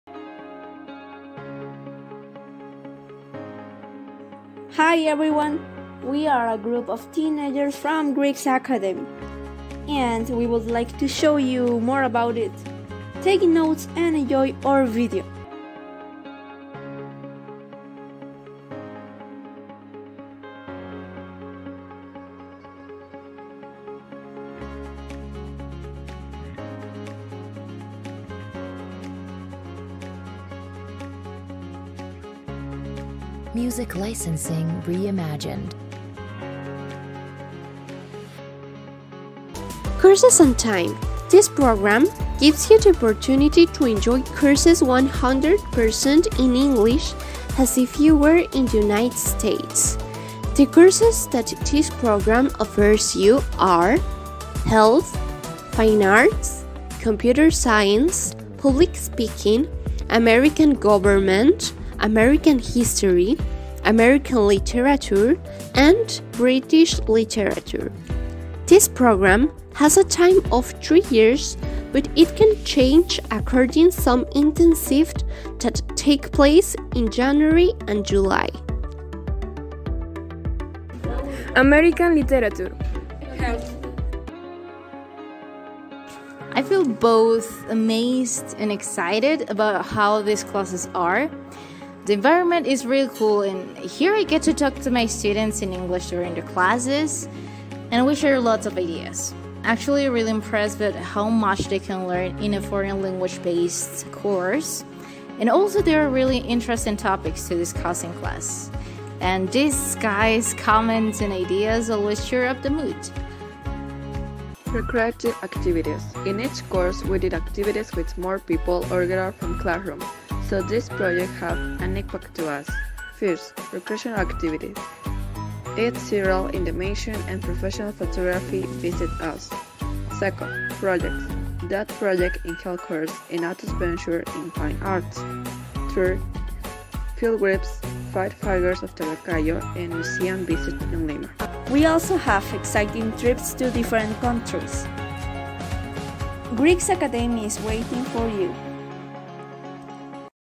GRIGGS ACADEMY PROMOTIONAL VIDEO
Hi, Friends! This video is our first project promoting Griggs Academy, we hope you enjoy it and you can understand the message, thank you for your attetion, see you later.